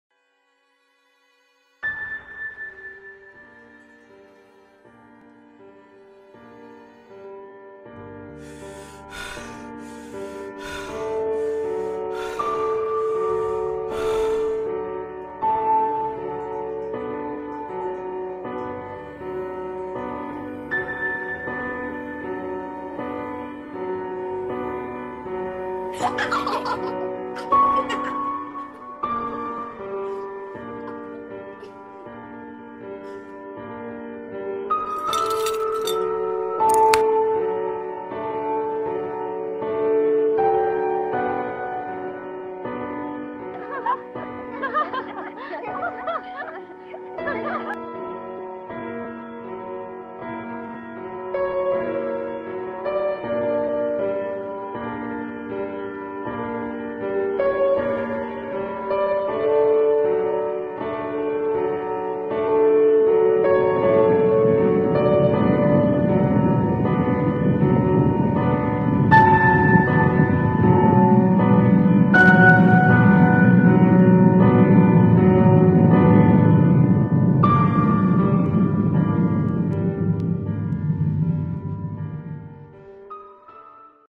Lebbeus Woods DMZ Reimagined
Animation based on artwork by Lebbeus Woods